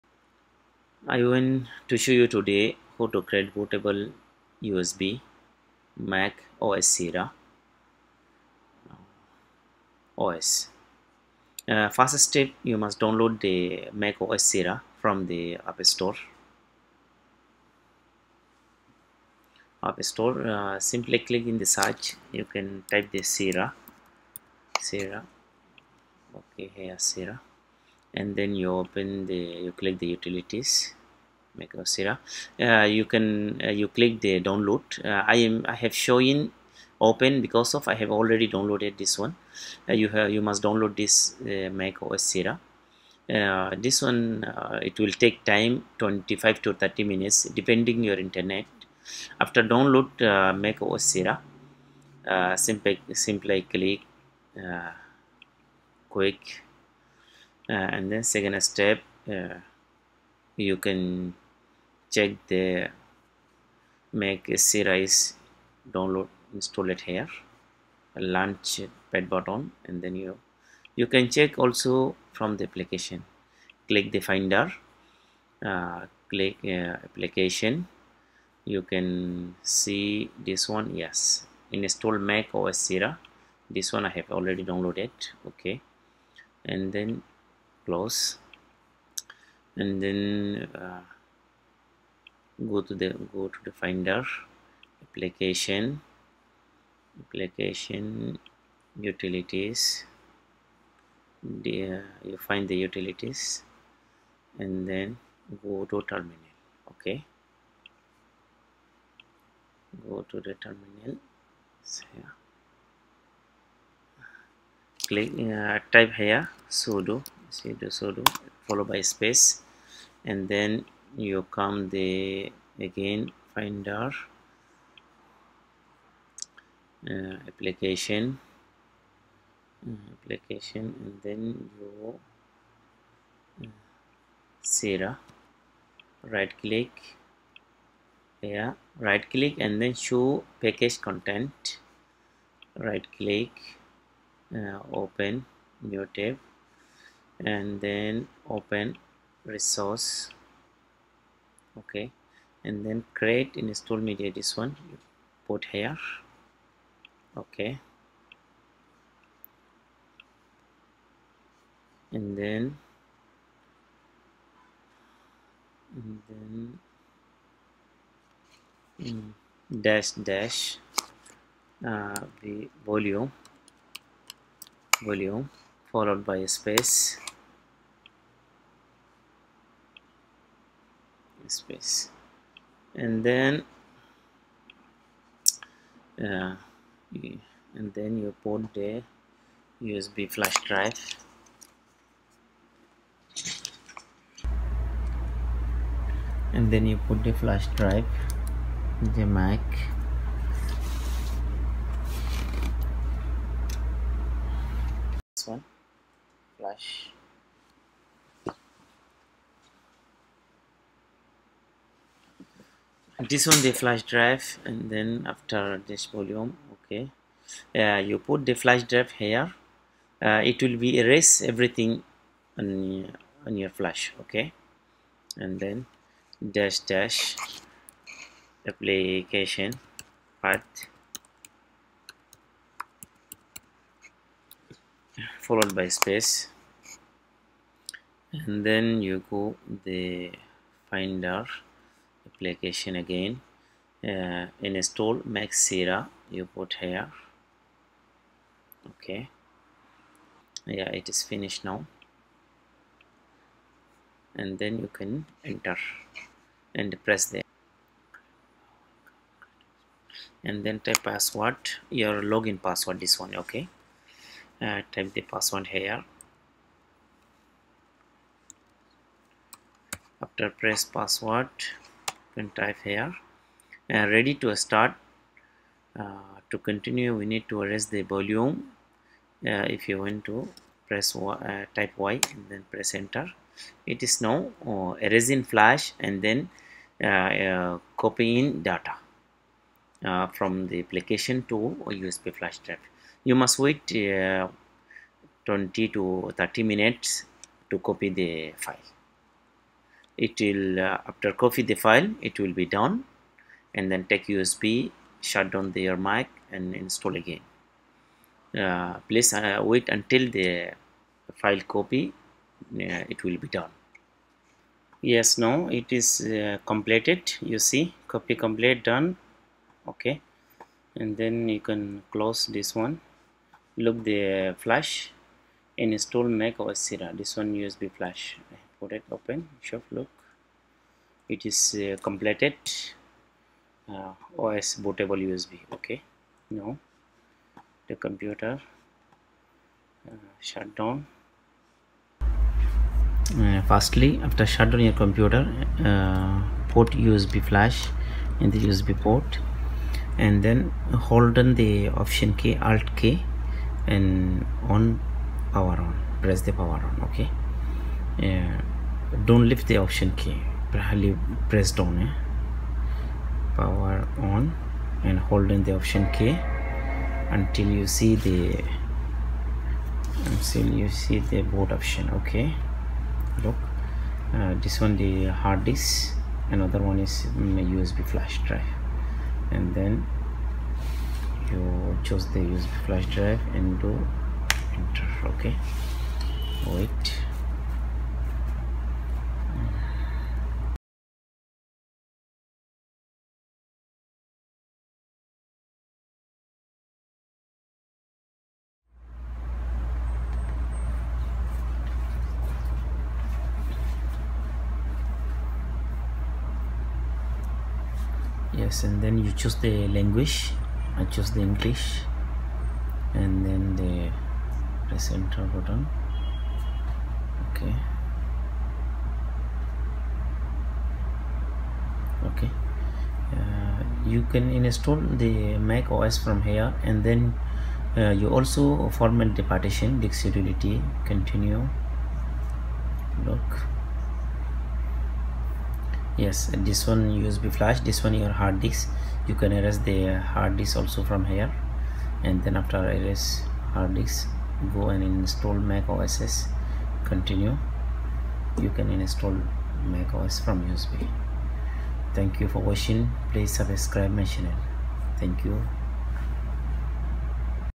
Create a Bootable USB Drive For MacOS High Sierra easy step by step
Description
In this video I'm going to show you "How to create a Bootable USB Flash Drive For MacOS Sierra" type manually command or copy & paste from here.

sudo /Applications/Install\ macOS\ High\ Sierra.app/Contents/Resources/createinstallmedia --volume /Volumes/TOSHIBA --applicationpath /Applications/Install\ macOS\ High\ Sierra.app

Welcome to Technical Arakan. I'm not gonna write long description, If you want to know more about channel then watch my videos. I hope you can find some useful videos for yourself.